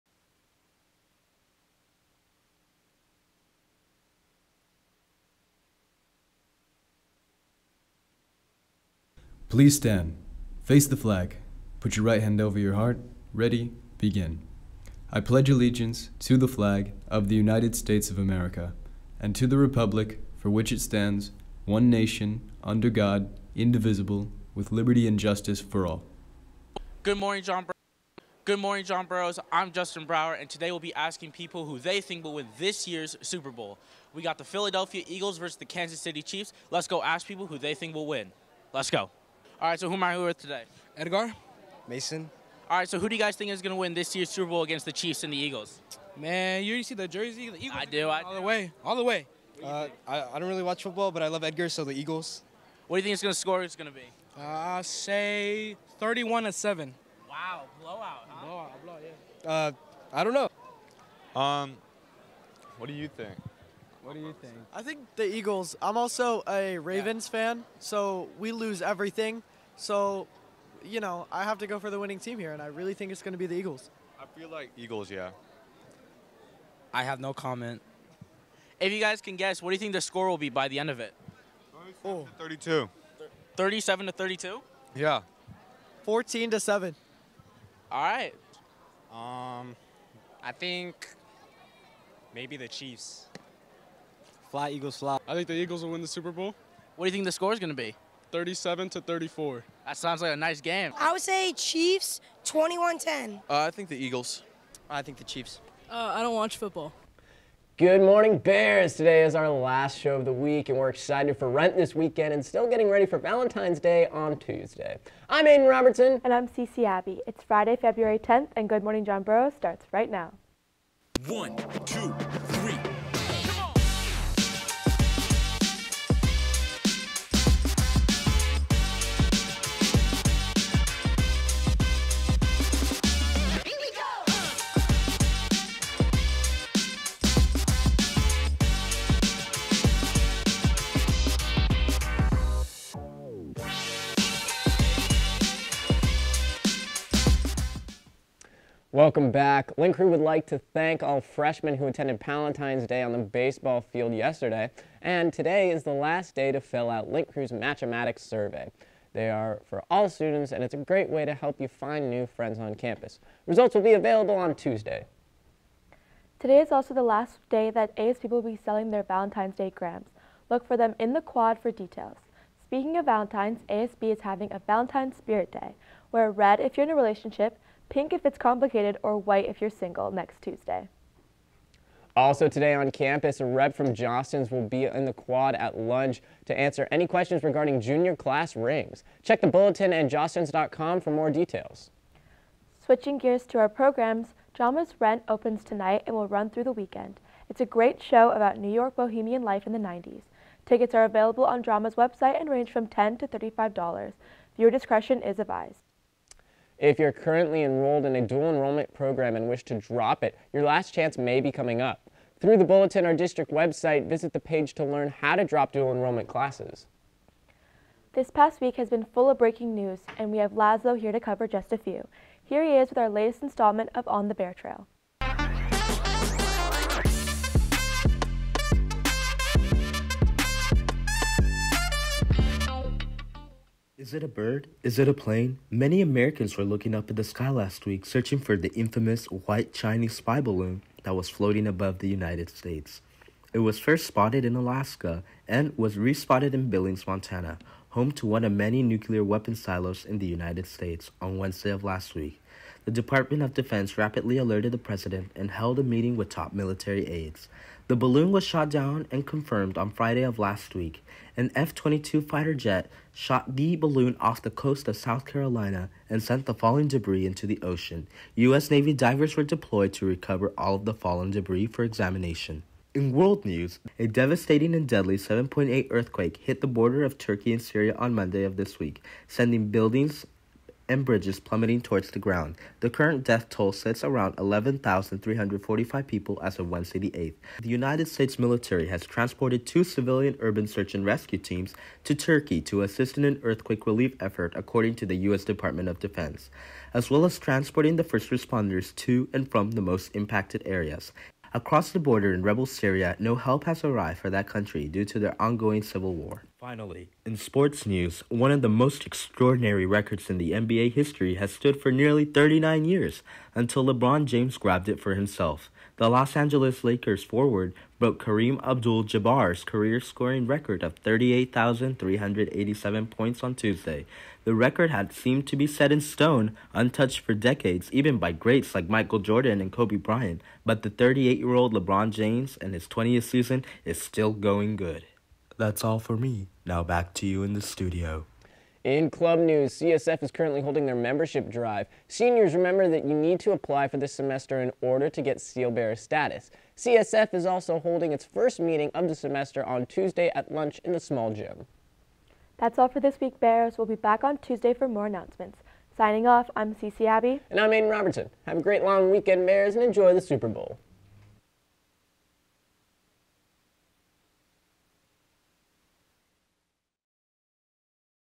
GMJB 2/10/2023
Anchors: Aidan Robertson, Cece Abbey

Segment: Man on the Street; Justin Brauer, On the Bear Trail; Lazlo Aivazian